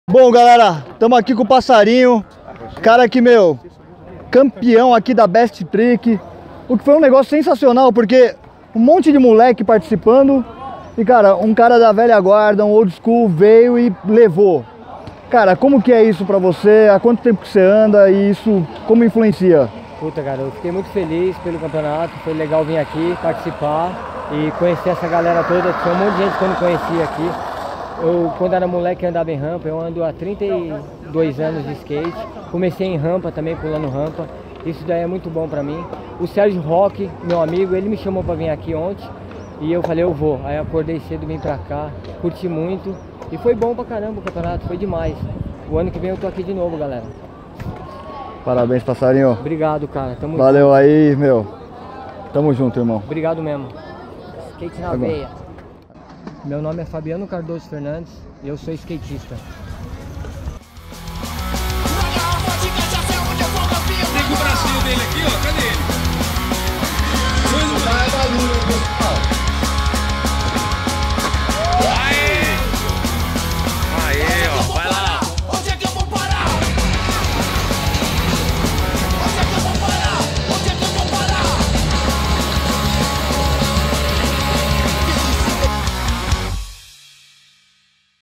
Desafio Eu Sou Skatista: Jump Ramp 2019 - Fabiano Fernandes "Passarinho"
Conheça o skatista oldschool que provou que não é idade que vence besttrick, mas sim skate no pé! E olha que apesar dele ter muito skate no pé, ele tem skate até na mão!!!